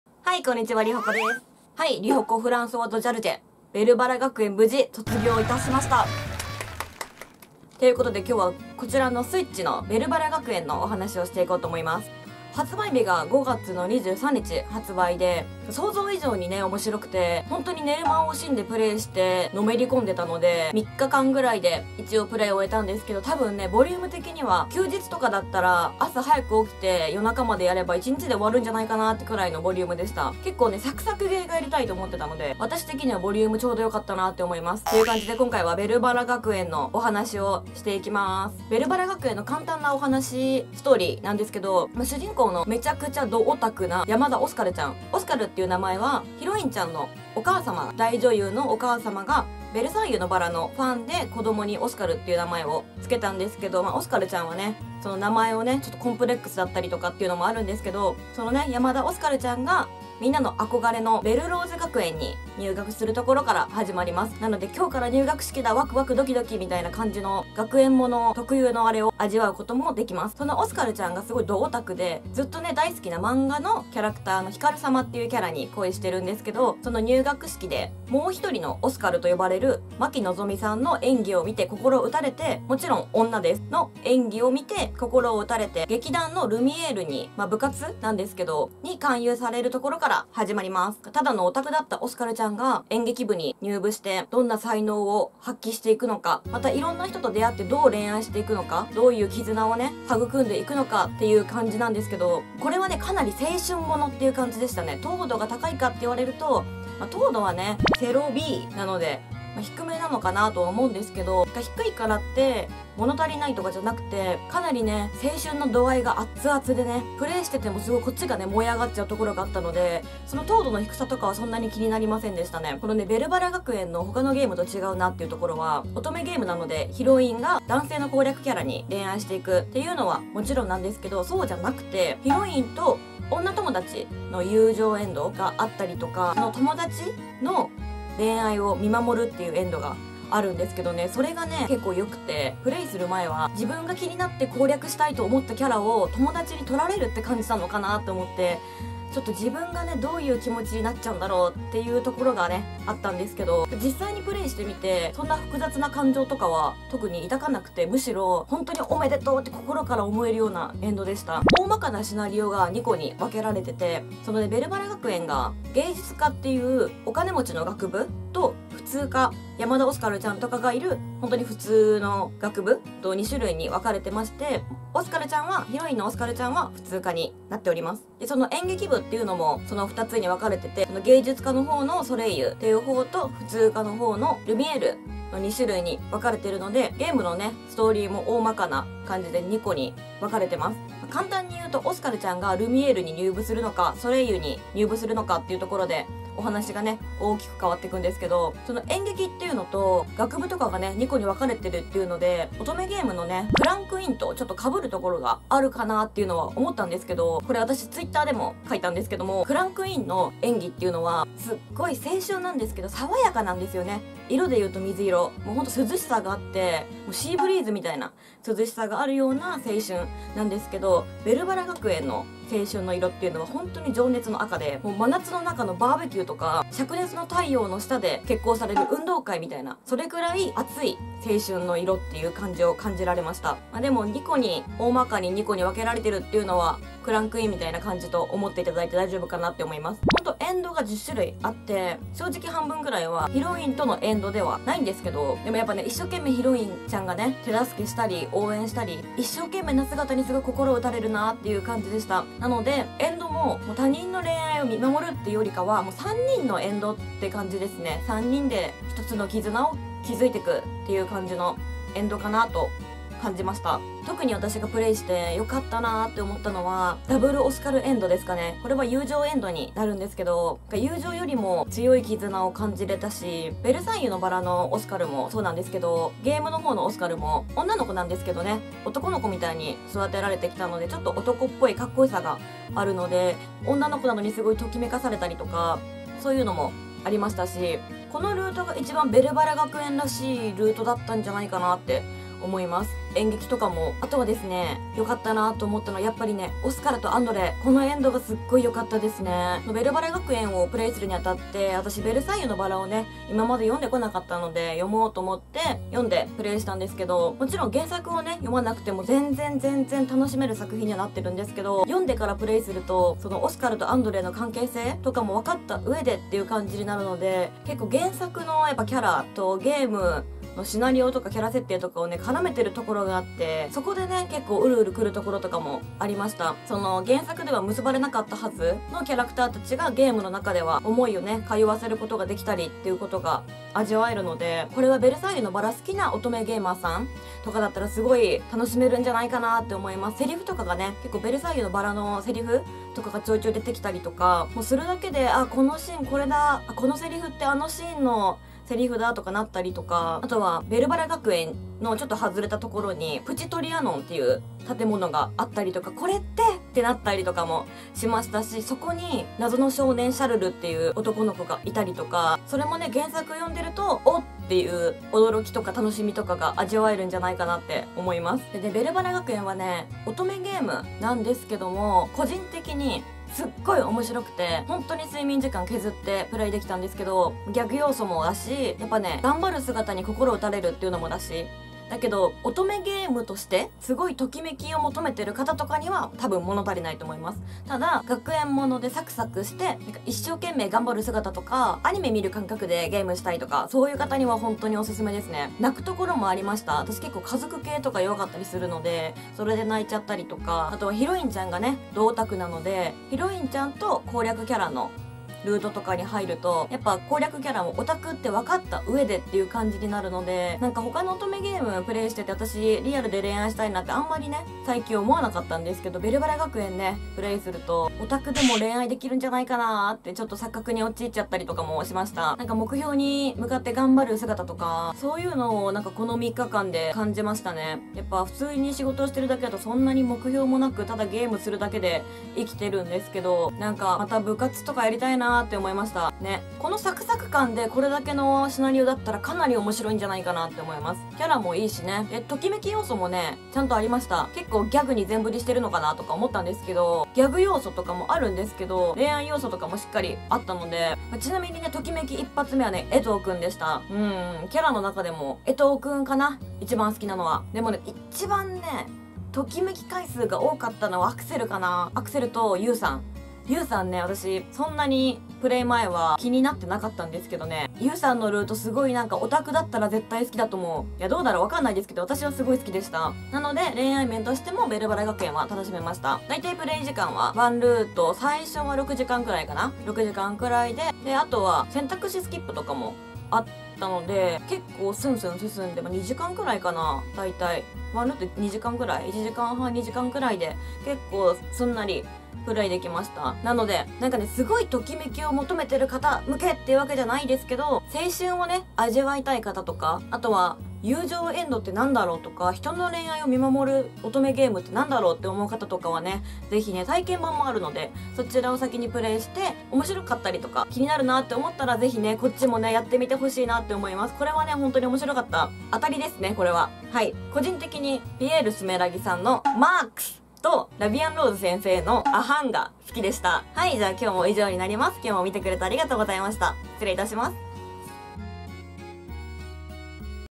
大人気名作少女漫画ベルサイユのばらの乙女ゲーム！？ベルばら学園プレイ感想
5月23日発売オトメイトさんの新作乙女ゲーム、ベルばら学園無事フルコンプしましたのでネタバレはなしでプレイ感想、紹介をします。
ギャグ・青春・恋愛のバランスがよかったですね！
ボリュームも程よくサクサクできた気がします

面白すぎて寝るの勿体なくて睡眠不足でクマが痛いのと肌荒れが絶頂期ですwww